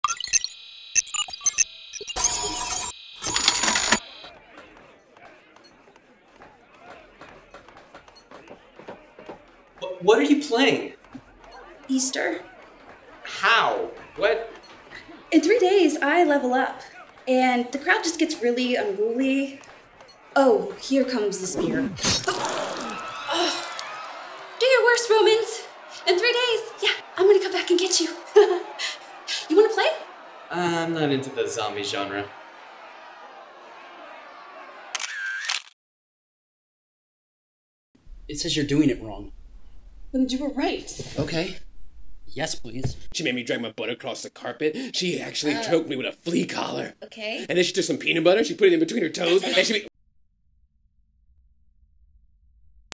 Easter - Game Room
Carly Anderson plays a grueling game of Easter while Mercer Ryan looks on.

Easter isn't available for the XBox 360 Kinect, Sony Playstation Move, or Nintendo Wii but it should be.